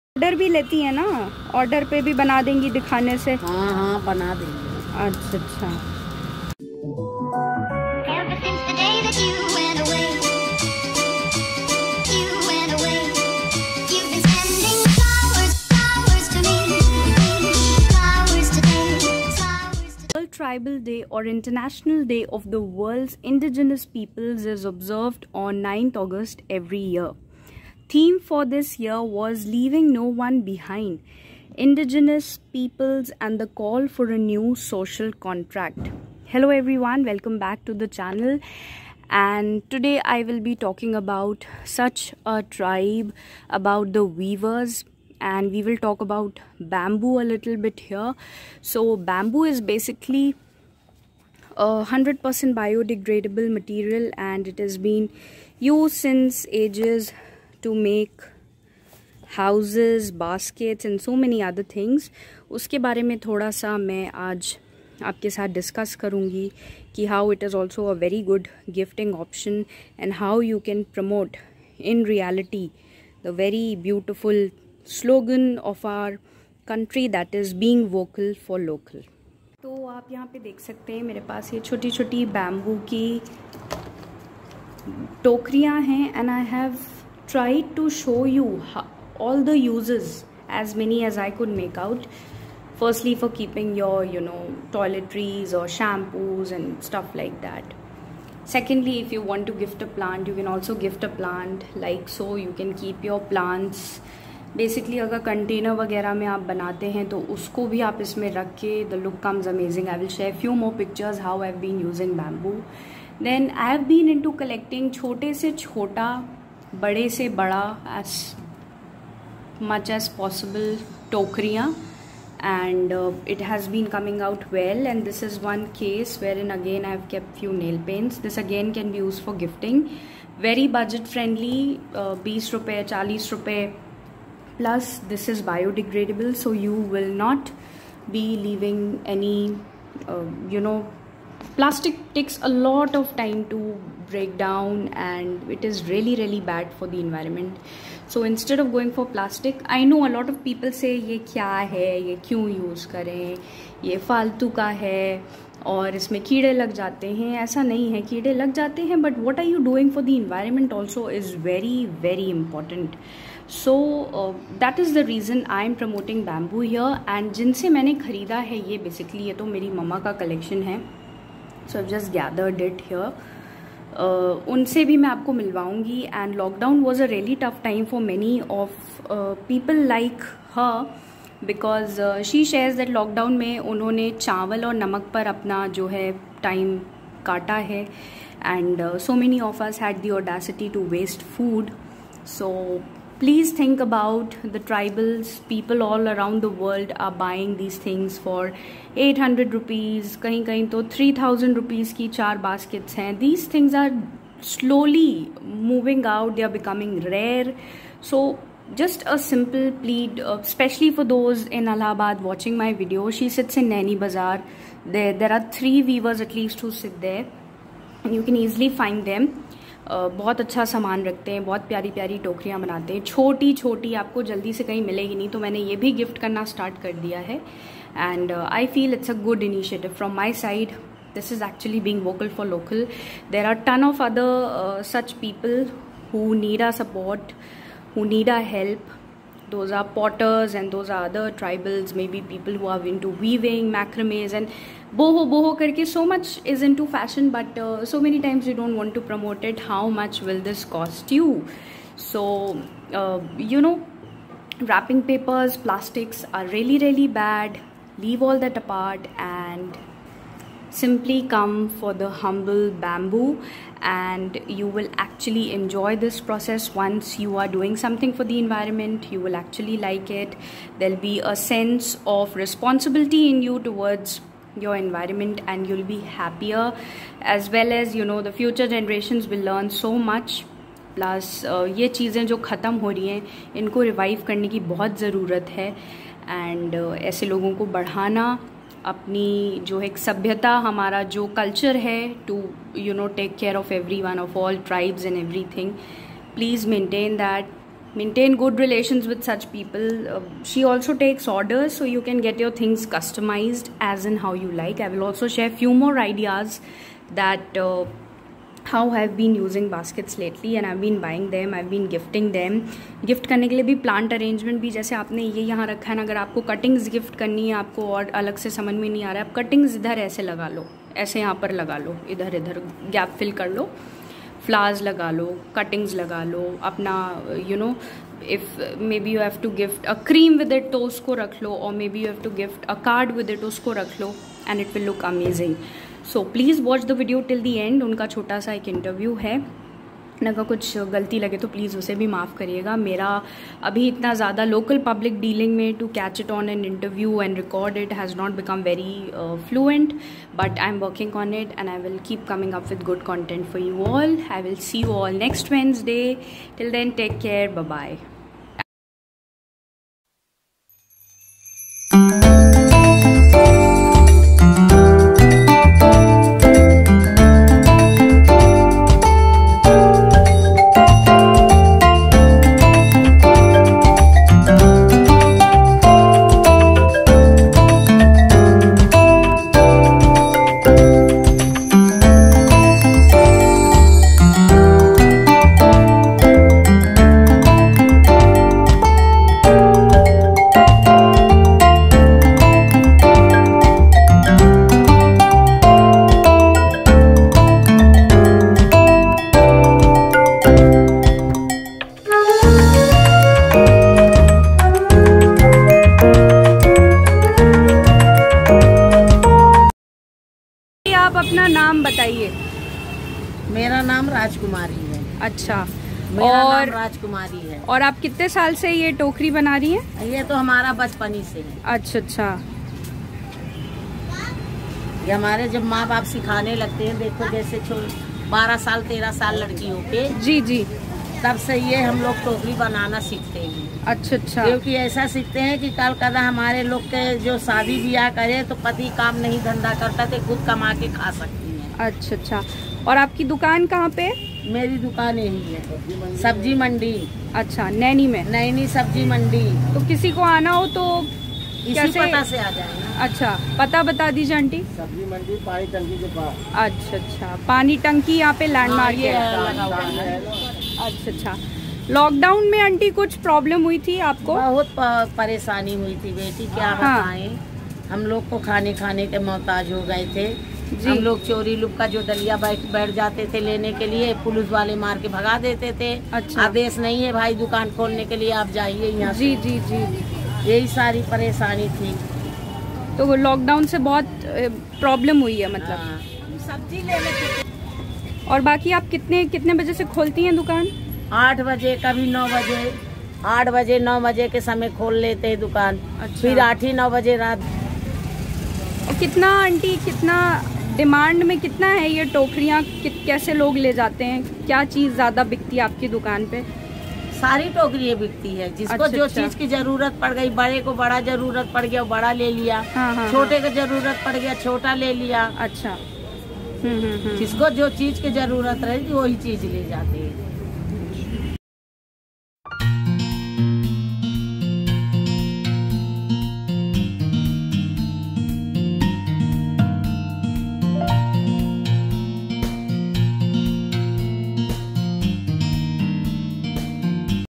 World Tribal day , a weavers tale ,the boho basket rare yet fashionable
Hello everyone,
Sharing with you all a little initiative I took recently switching to baskets which are 100 per cent biodegradable and is also preferred as a raw material by many industries over any other material because the farming is also not dangerous for our environment anyhow.Stay green be vocal for local.This video is coming late but looking forward for your love as always.
Jessica
Allahabad walo you can find her in Naini sabzi Mandi ,near Jail road.